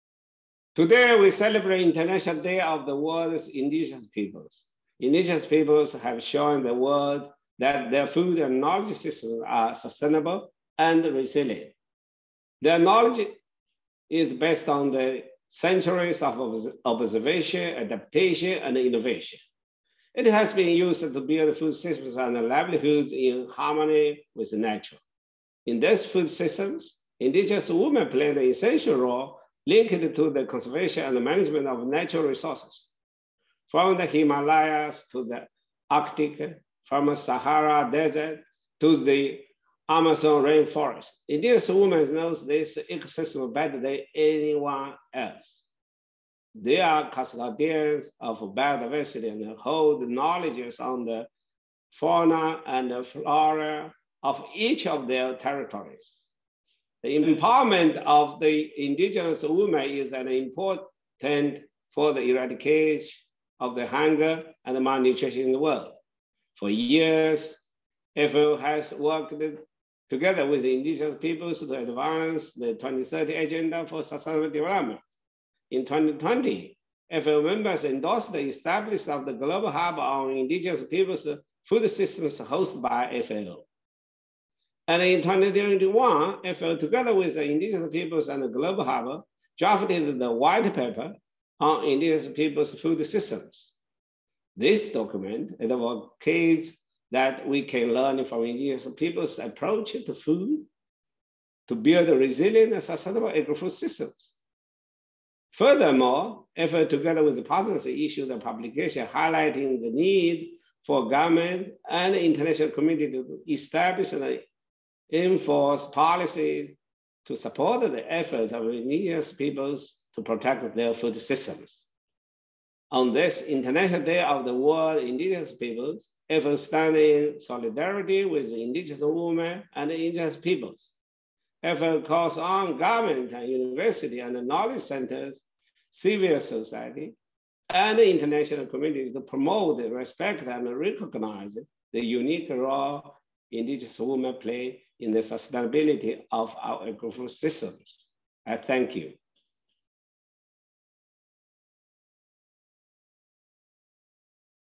FAO Director-General QU Dongyu for the International Day of the World’s Indigenous Peoples 2022
On August 9th, the International Day of the World's Indigenous People is commemorated.  This year, the day focuses on the theme "The Role of Indigenous Women in the Preservation and Transmission of Traditional Knowledge.
Indigenous Peoples have shown the world that their food and knowledge systems are sustainable, resilient and can contribute to the global debate on climate action and sustainable agri-food systems. However, Indigenous Peoples are currently facing the major impacts of climate change, although they contribute the least to global warming. FAO has joined forces with Indigenous Peoples around the world to promote the recognition, respect and strengthening of Indigenous Peoples' food and knowledge systems.